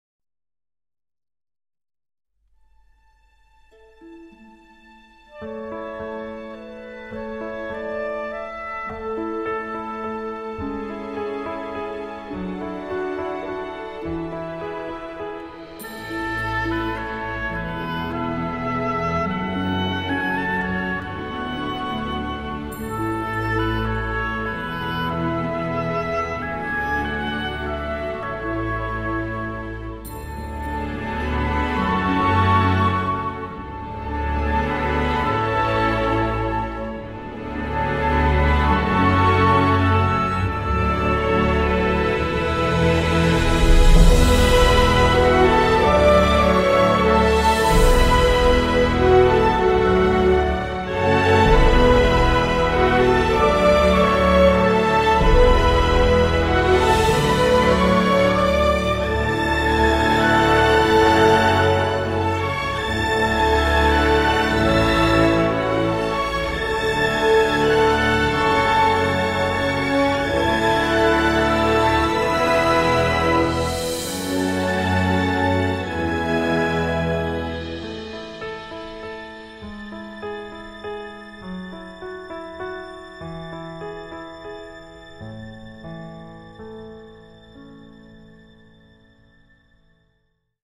Gross Und Klein Trailer
Botho Strauß
Inszenierung: Joe Knipp; Bühne und Kostüme: Hannelore Honnen
Abschlussklasse der Theaterakademie 2012
Akademietheater Köln